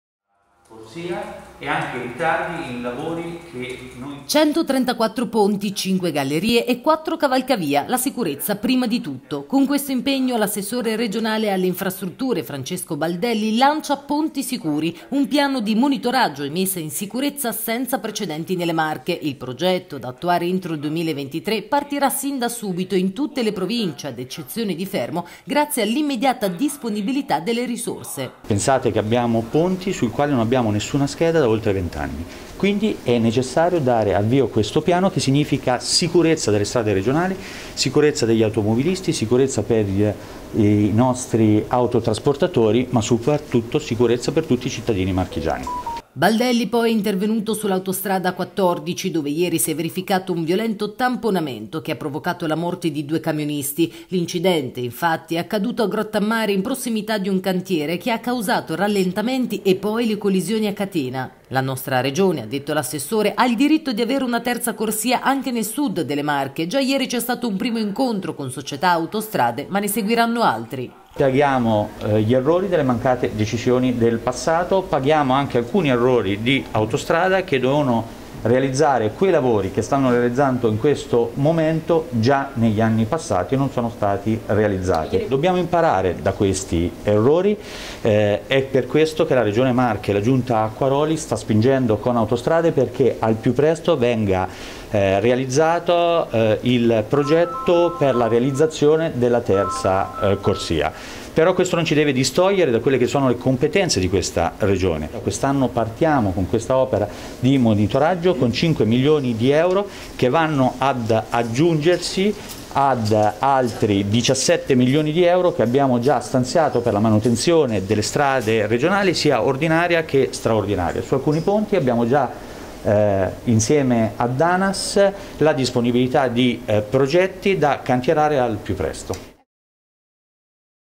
Ponti sicuri, Baldelli: "Stanziati 5 milioni"
La Regione Marche ha stanziato 5 milioni di euro per un piano di monitoraggio e di manutenzione senza precedenti sulla viabilità. Ad illustrare il progetto, denominato “Ponti Sicuri” è l’assessore Francesco Baldelli.